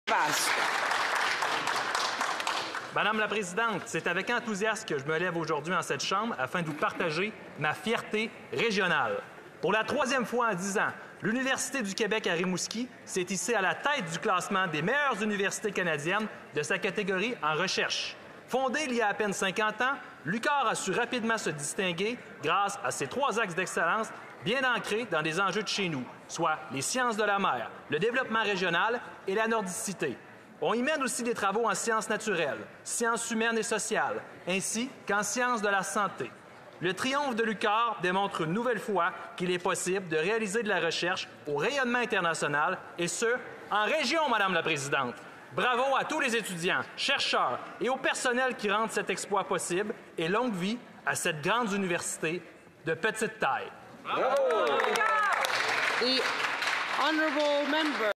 L'Université du Québec à Rimouski : une université de haute prestige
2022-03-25 - Déclaration des députés à la Chambre des communes le 25 mars 2022.

Le député Maxime Blanchette-Joncas félicite l'Université du Québec à Rimouski qui se qualifie pour une troisième fois en dix ans s'est placé à la tête du classement des meilleures universités canadiennes de sa catégorie en recherche. Félicitations à l'UQAR pour cette distinction!

Autorisé par l'agent officiel de Maxime Blanchette-Joncas